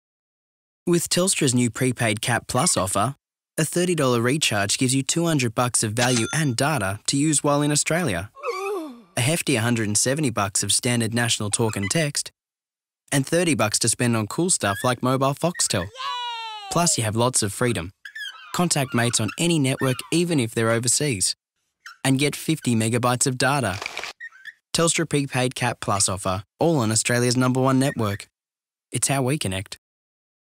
Telstra Commercial 30 Second Version - Drawn by Jeremy Ley / Directed by Josh Logue
The 30 second version of the Telstra Pre-Paid Cap Offer commercial. This version aired on Australian television for about a month. I suddenly got a lot of calls, texts, and messages from people I hadn't seen in a while. "Hey man. Did you do that Telstra ad? I swear it was you."

The advertisement was drawn live on a whiteboard by Jeremy Ley, a freelance illustrator and storyboard artist. It was filmed and directed by Josh Logue and his talented gang. The Feds were the production house that put this all together and DDB were the advertising agency that dreamed up the fantastic idea.